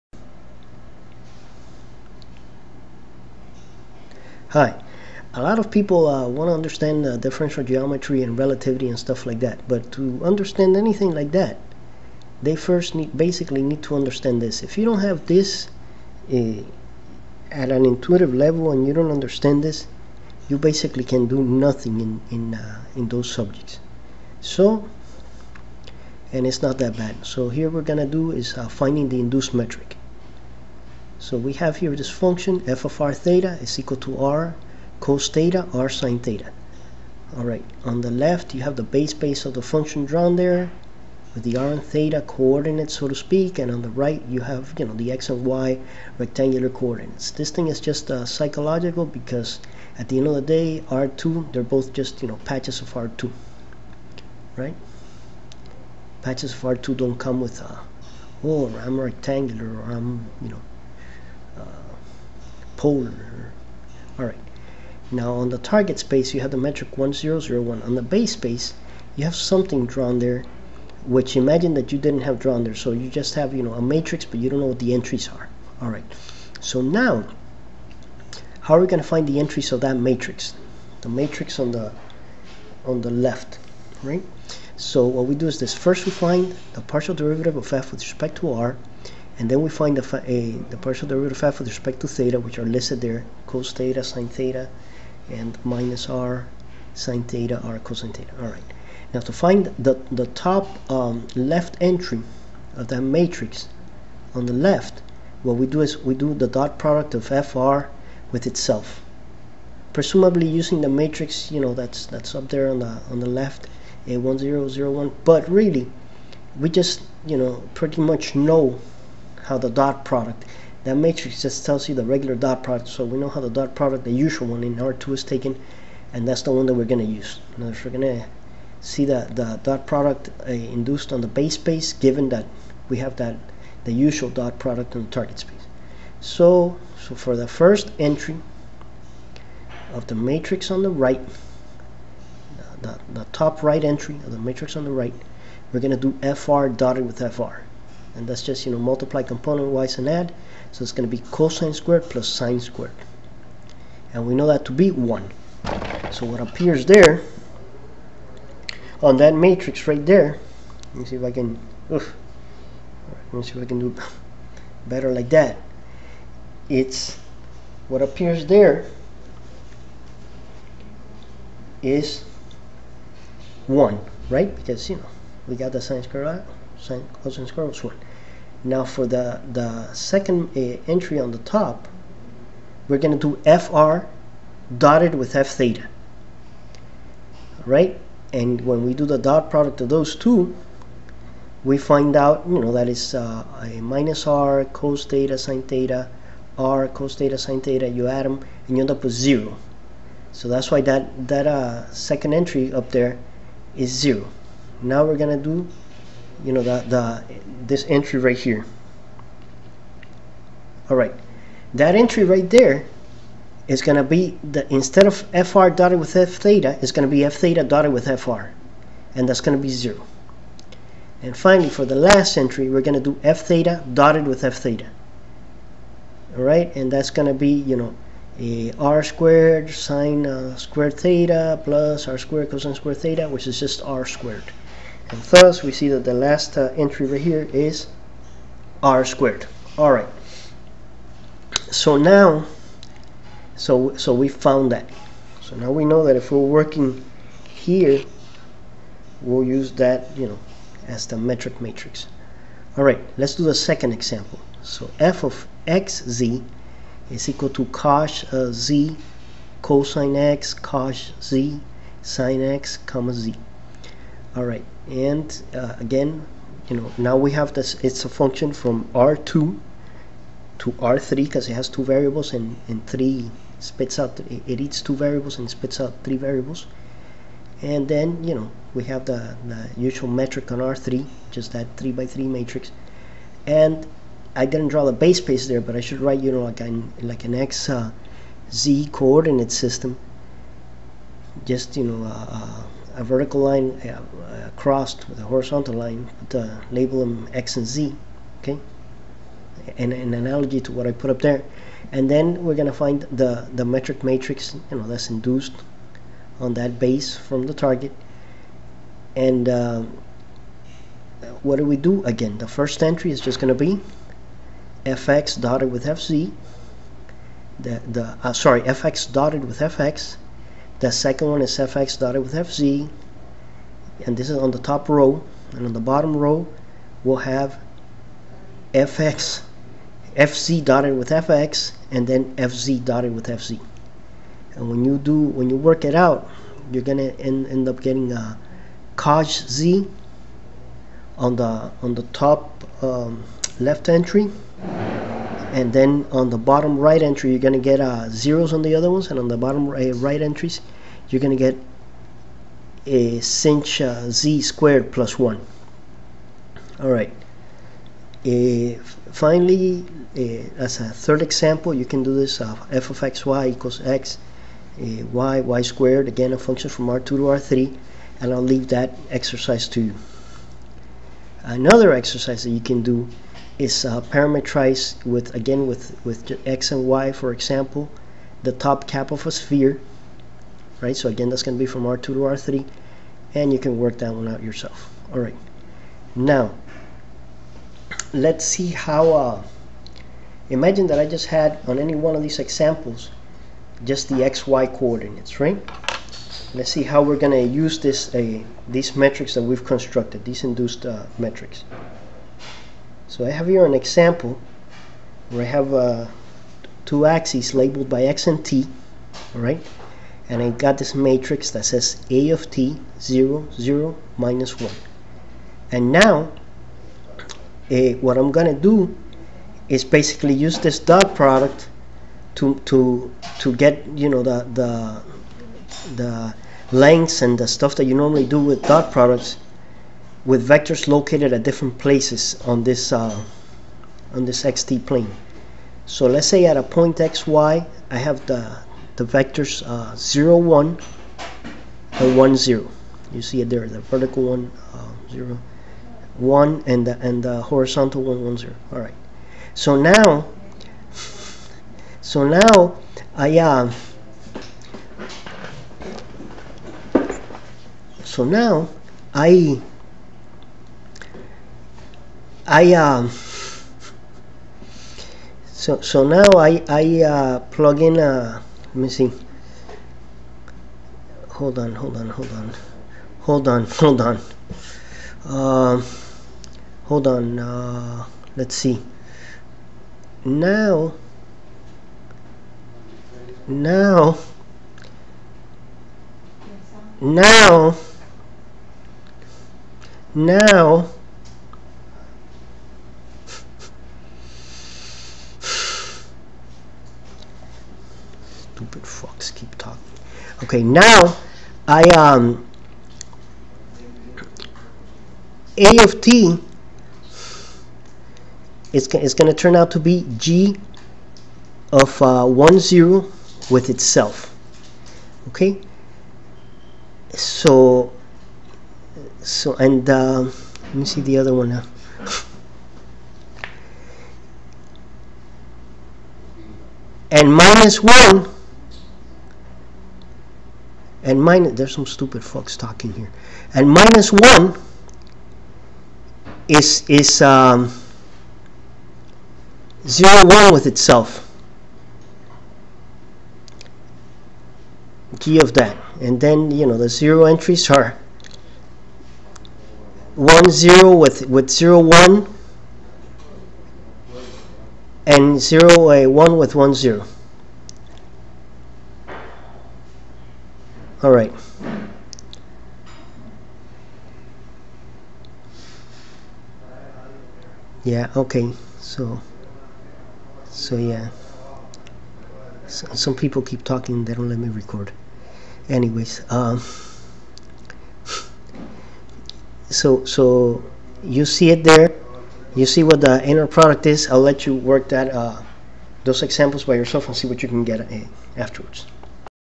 Finding the Induced Metric
Finiding the metric induced on the base or domain of a fumction
by the metric on the target space (in  these example the id matrix)
though in general the matrix on the target space is arbitrary.
Then I begin to show you how to use it.